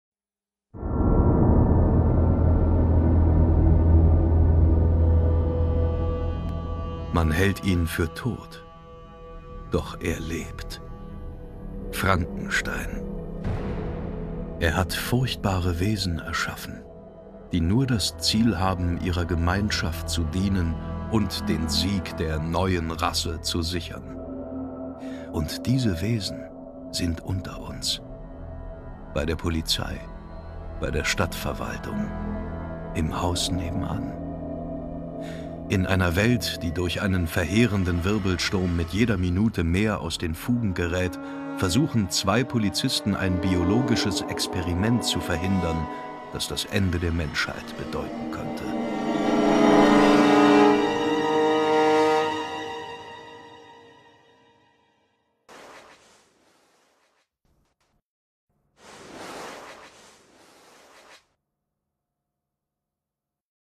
Dean Koontz - Der Schatten (Frankenstein 3) - Hörbuch
Trailer von Audible.de zum Hörbuch "Der Schatten - Frankenstein 3" von Dean Koontz gelesen von David Nathan. Dieses Hörbuch gibt es bei Audible exklusiv und ungekürzt zum Download.